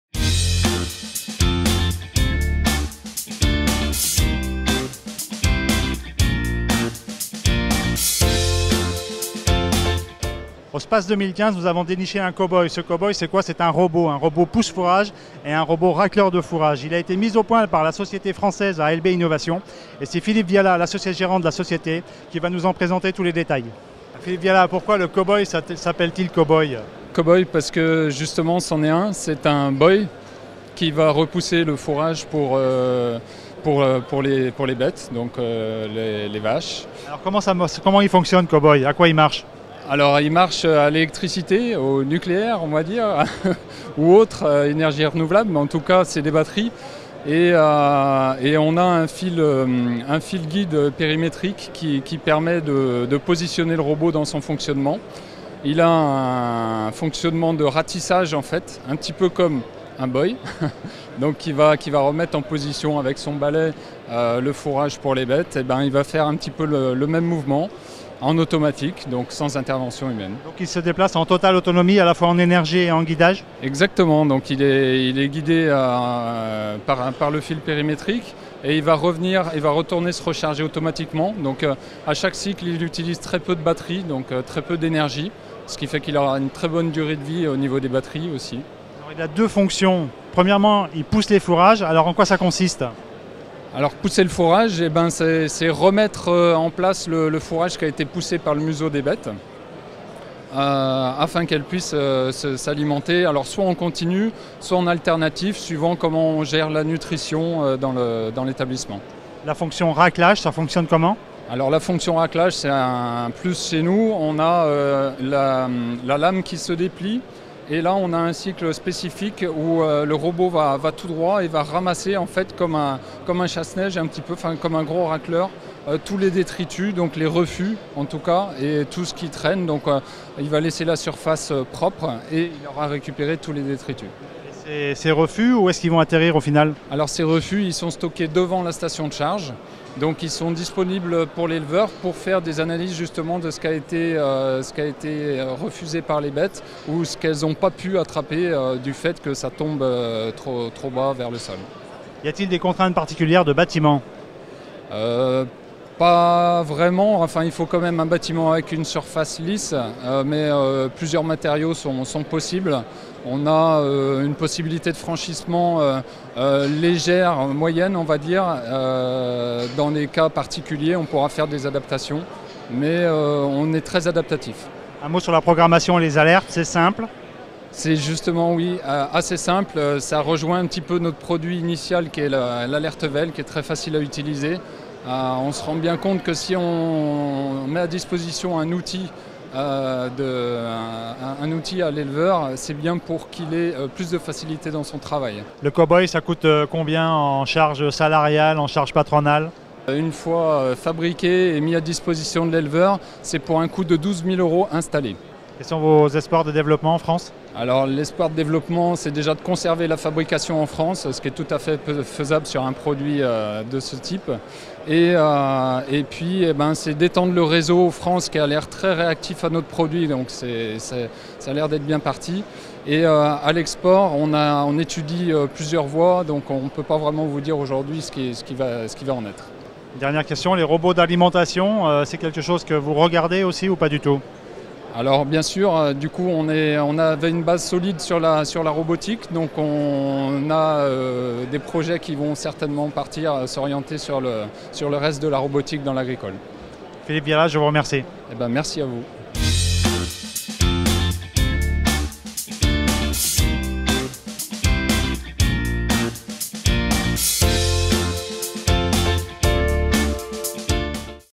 Space 2015 : Chapeau au Cow-boy d’ALB Innovation
Monté sur batterie et doté d’un filo-guide en guise de lasso, le Cow-boy cumule les fonctions de pousse-fourrages et de racleur de refus. Innov-Space 2015.